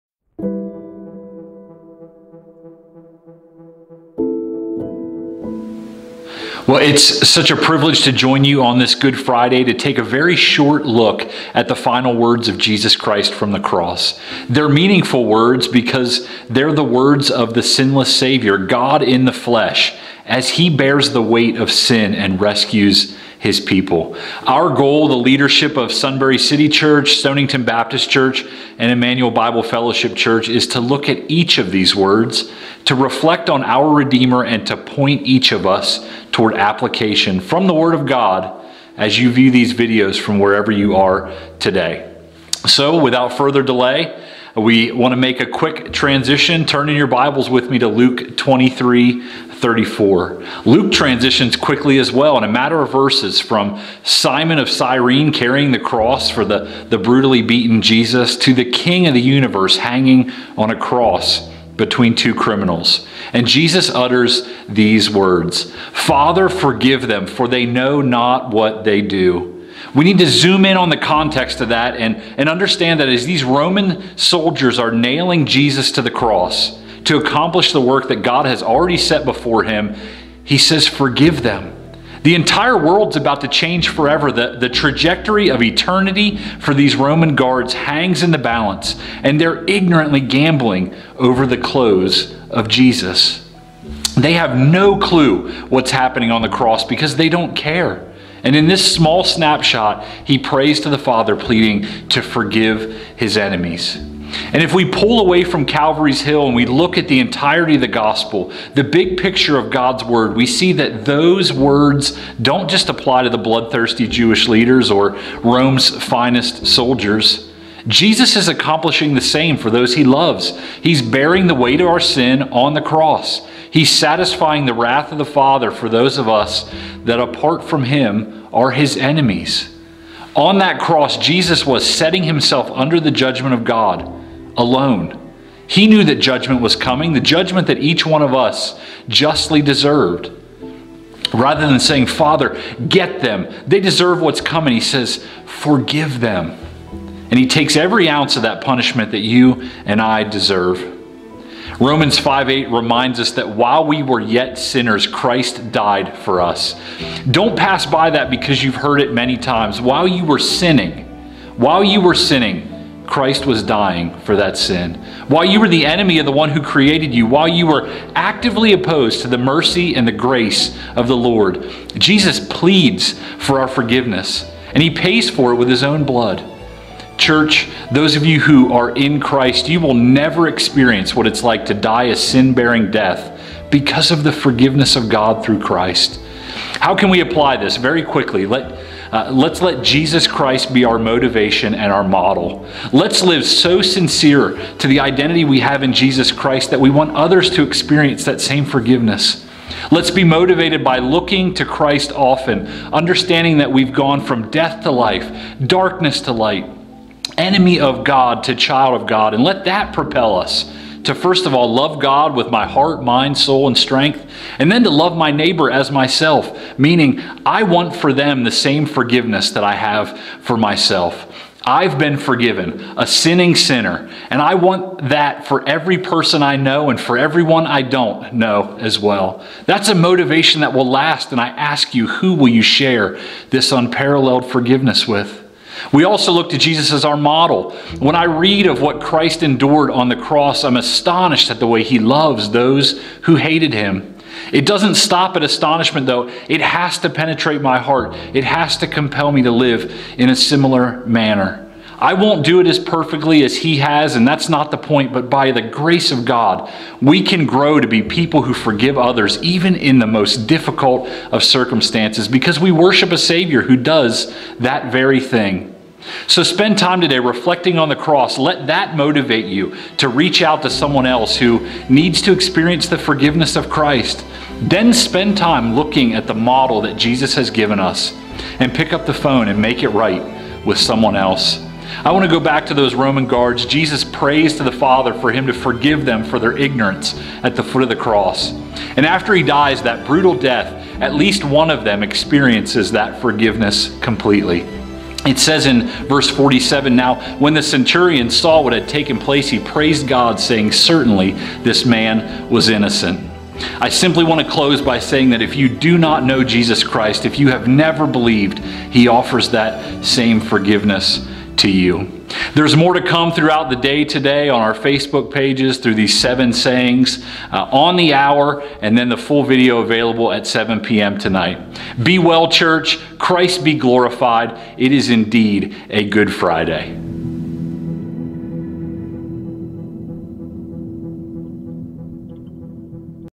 Seven Sayings - Father, forgive them
The leadership of Sunbury City Church, Stonington Baptist Church, and Emmanuel Bible Fellowship Church walk through the seven last sayings of Jesus Christ from the Cross. These videos will be posted on the hour from 9am - 3pm on Good Friday and the full video will be posted at 7pm.